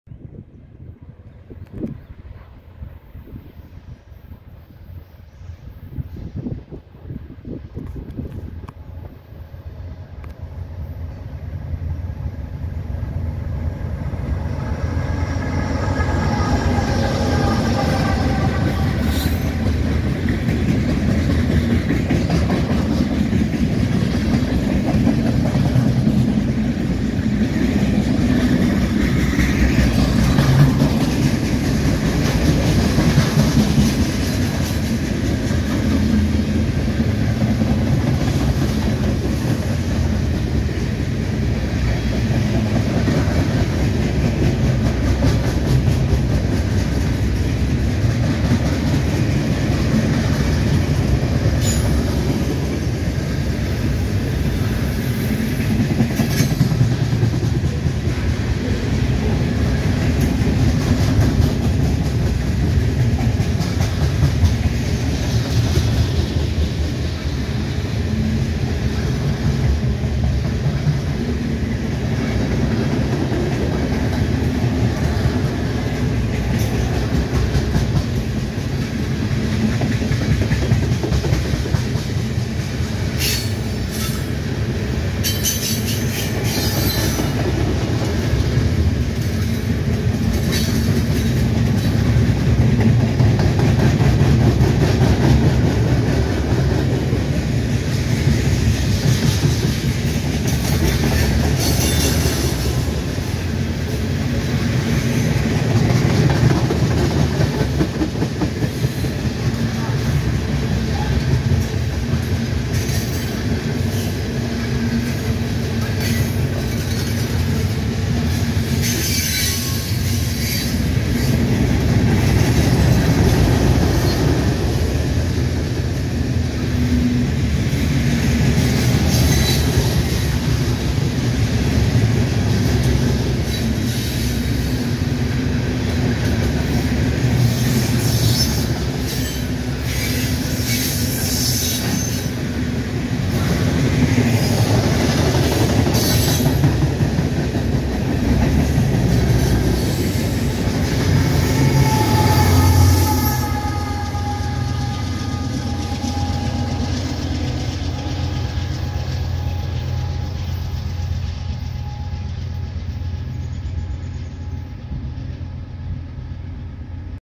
Eastbound NS Manifest 1 3-13-2024
at 12:04pm, the first train in the afternoon came past. lost of interesting rolling stock can be seen here.